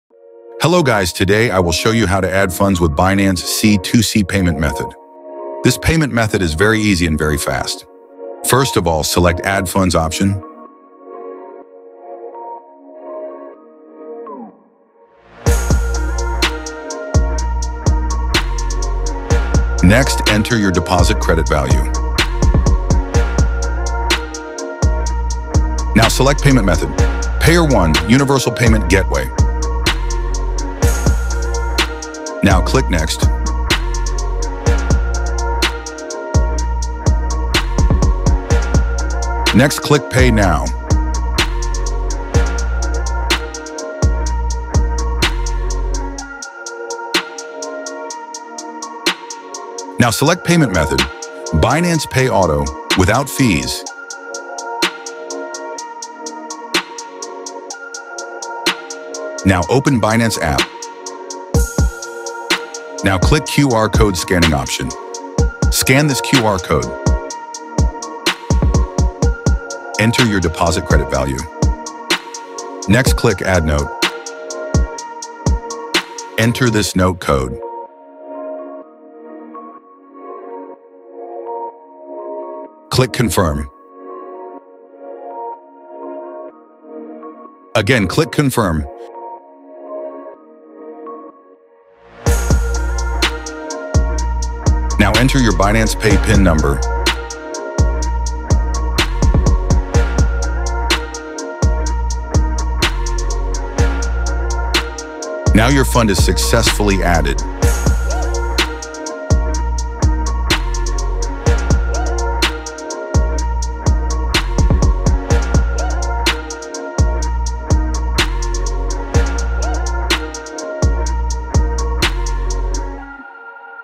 Auto Funds Add Anytime with 0% fees in Unlocktrue Server | Binance C2C Step-By-Step
How to Auto Add funds to Binance Pay C2C 0% Fees| Step-by-Step Guide